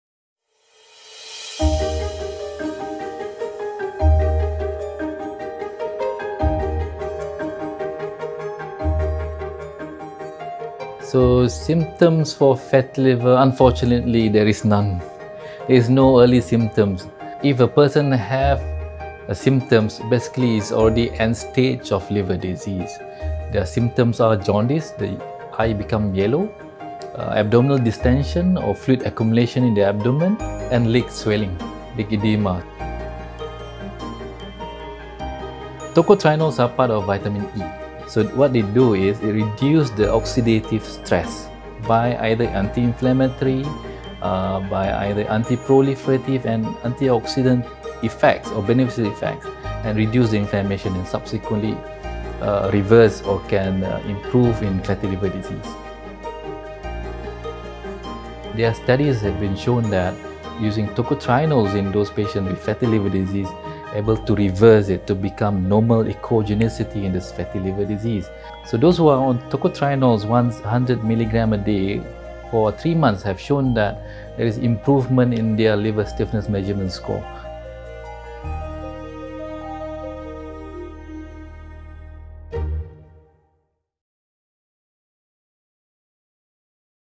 Apa itu Tokotrienols daripada Dr Ahmad Najib Azmi
Tokotrienol sebenarnya berasal daripada keluarga  Vitamin E dan fungsi  utama Vitamin E adalah mengurangkan keradangan organ dalam badan.

Perkara ini jelas menunjukkan Tokotrienol wujud untuk memastikan pesakit hati berlemak mempunyai peluang menjalani aktiviti harian seperti biasa.

Temui Dr Ahmad Najib Azmi untuk penerangan lanjut mengenai mengapa masalah hati berlemak ini berbahaya dan cara untuk mengawal masalah ini.